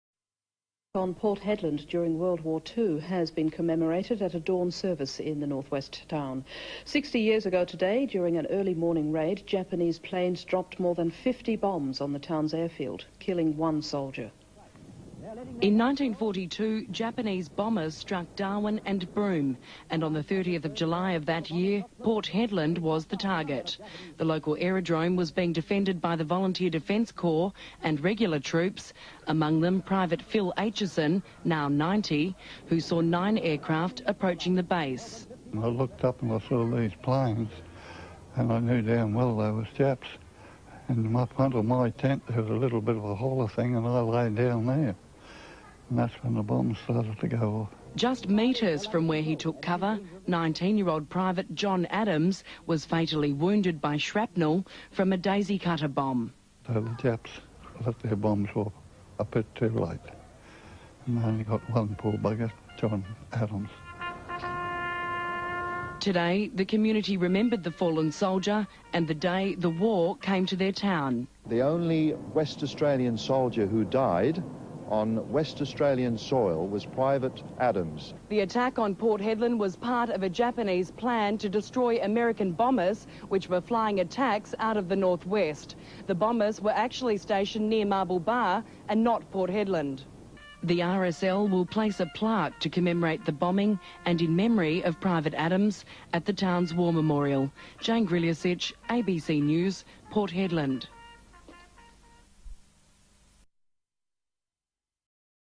Port Hedland World War II Attack commemorated 2002 - Jane Grljusich ABC News
80 years ago, on July 30, WWII came to Port Hedland, WA in the form of Japanese bombers looking to destroy an Allied staging post - they dropped “daisy cutter” bombs on the then airstrip of Port Hedland, fatally injuring one Private John Adams. The Japanese hit the wrong target.

This archival footage from ABC News is from twenty years ago.